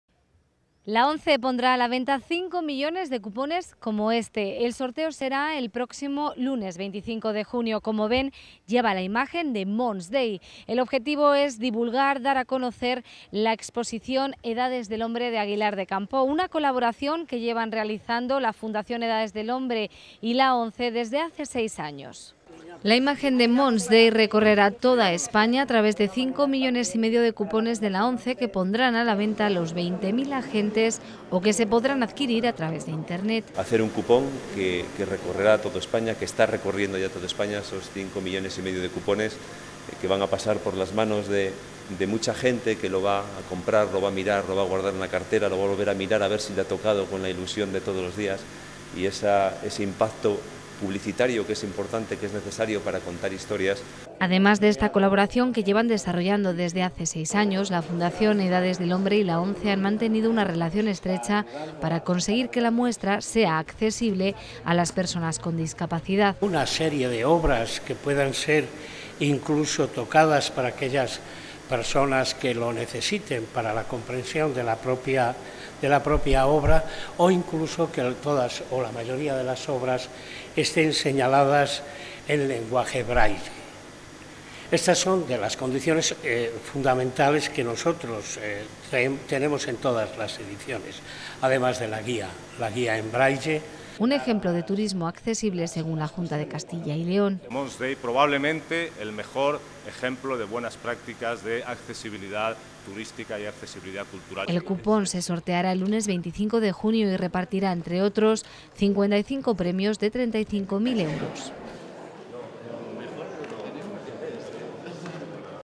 Mons Dei en el cupón
La exposición Edades del Hombre será la imagen del cupón de la ONCE del lunes 25 de junio. Una forma de publicitar Mons Dei y de fomentar la colaboración entre las dos Fundaciones para garantizar la accesibilidad de la muestra a las personas con discapacidad.